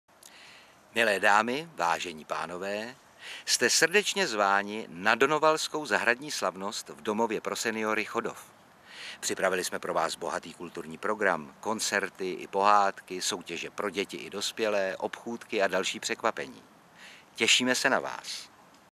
Pozvánka na Donovalskou zahradní slavnost 2
V pořadí již III. Donovalská zahradní slavnost, jejímž pořadatelem je Domov pro seniory Chodov, se uskuteční 9. září 2017. V době od 10 do 18 hodin jste zváni na bohatý kulturní program, kdy vystoupí např. Magda Malá nebo Daniel Hůlka. Pro děti jsou připraveny celodenní soutěže o ceny, malování na obličej a dvě divadelní představení pořádaná sdružením FysioART, o.p.s.

V rámci doprovodných akcí pro vás spolek Bosá turistika připravil „bosonohý program“, kde si mmj. můžete projít naši bosonohou stezku. Dále bude možnost pro malé i velké tvoření v kreativních dílnách, osvojení si prvků z haptických masáží pro seniory (a nejen pro ně) a třeba si i vybrat knihu na burze knih. Samozřejmostí jsou obchůdky a výborné občerstvení přímo v areálu DS Chodov.

V letošním roce bude nově příležitost získat bližší informace o pracovních možnostech v DS Chodov v náborovém stánku aneb „Hledáme superhrdiny“.

Přijměte pozvání od vedení Domova, klientů a zaměstnanců na 9. září 2017, abyste na vlastní oči viděli, že život v domovech pro seniory pokračuje dál, jen ve změněných kulisách. Pomozte sejmout stigma z pobytových zařízení nebo se jen tak přijďte v sobotu pobavit se svými dětmi.

Celá akce se koná pod záštitou Magistrátu hl. města Prahy a Městské části Prahy 11.

PROGRAM

10:00 Zahájení

10:30 Magda Malá

12:00 Hardy a Šimi

14:00 DANIEL HŮLKA

16:15 Dlouhá noc

Událost na Facebooku: 1url.cz/oth8X

Autorka videa: Hana Strejčková
Herec: Otakar Brousek ml.